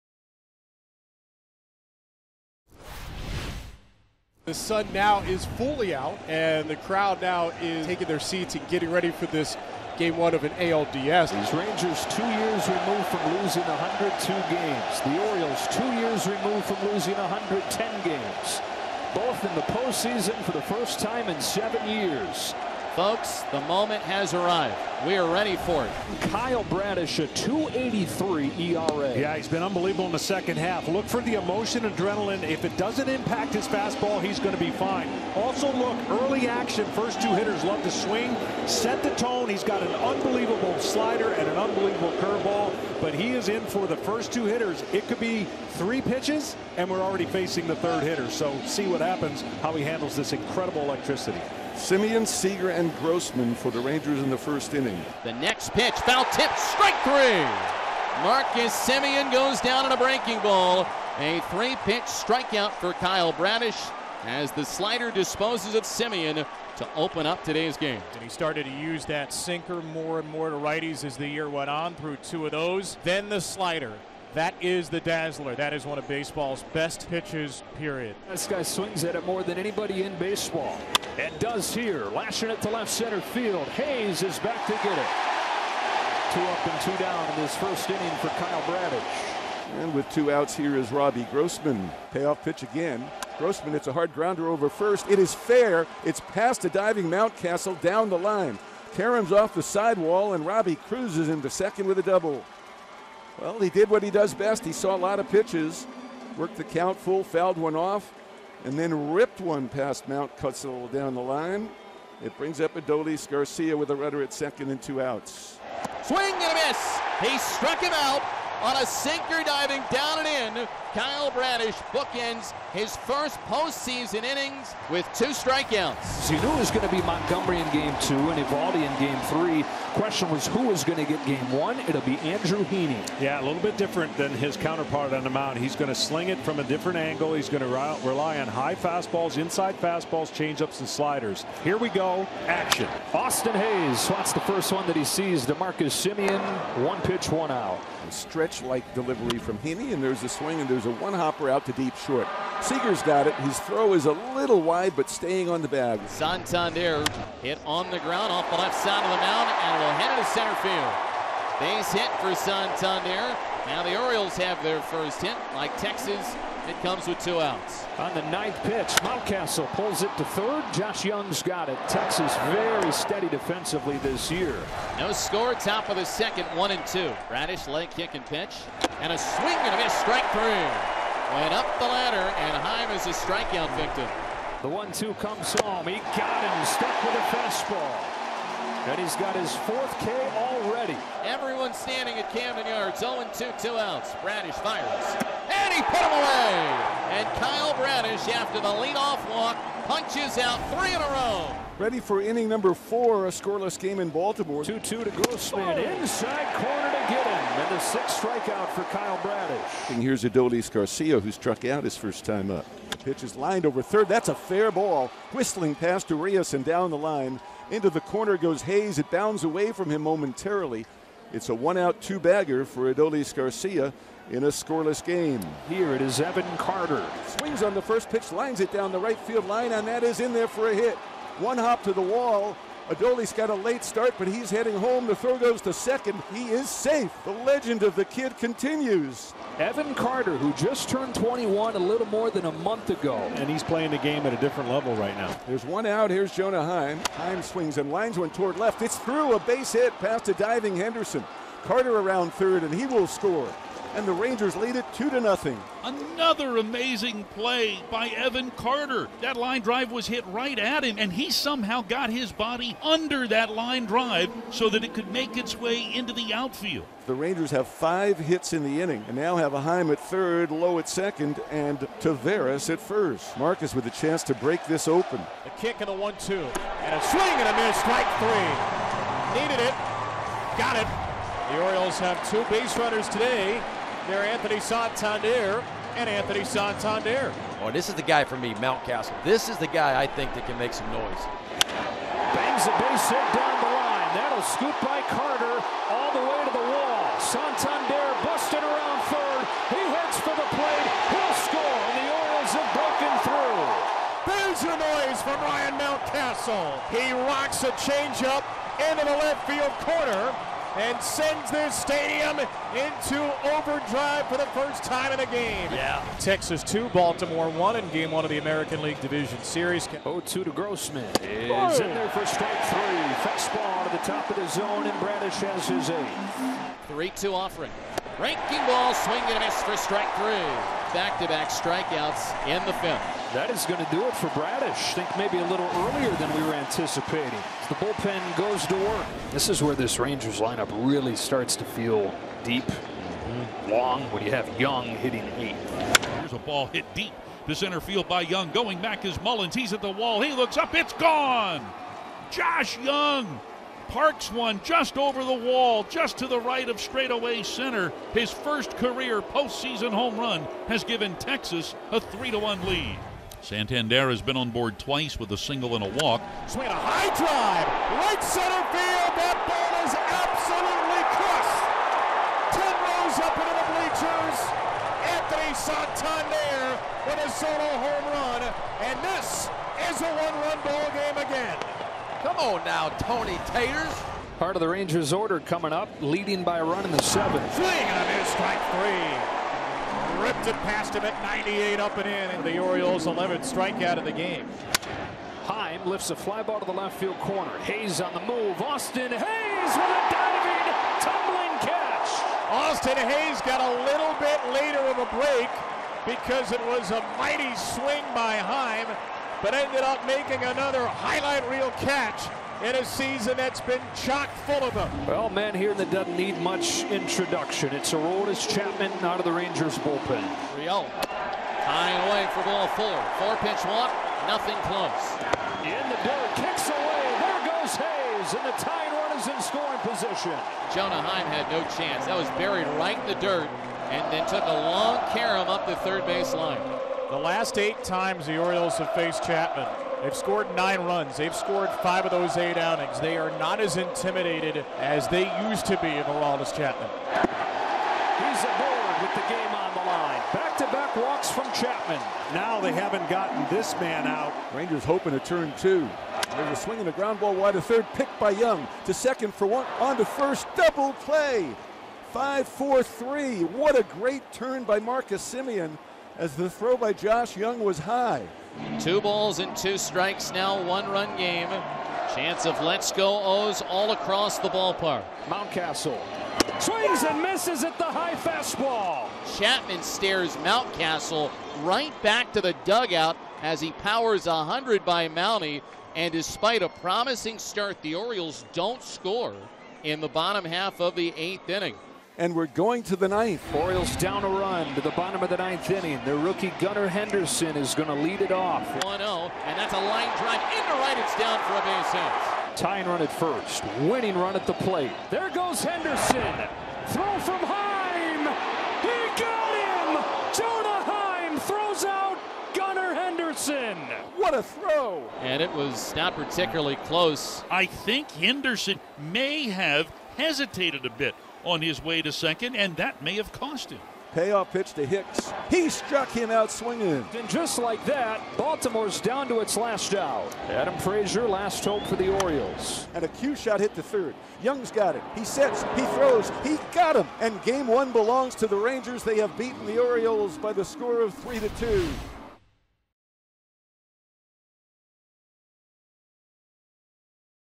Rangers vs. Orioles ALDS Game 1 Highlights (10/7/23) | MLB Highlights
Rangers vs. Orioles ALDS Game 1 full game highlights from 10/7/23, presented by  @evanwilliamsbourbon

(1:30) Robbie Grossman doubles for the Rangers' first hit of the ALDS
(3:30) Adolis Garcia doubles into left field
(3:46) Evan Carter doubles to right field to drive in Garcia
(4:51) Ryan Mountcastles drives in Anthony Santander with a double
(5:43) Josh Jung hits a solo home run to center
(6:03) Anthony Santander pulls the Orioles within 1 with a solo
(7:32) Aroldis Chapman induces a double play to end a scoring threat in the 8th
(8:51) The Rangers secure a Game 1 win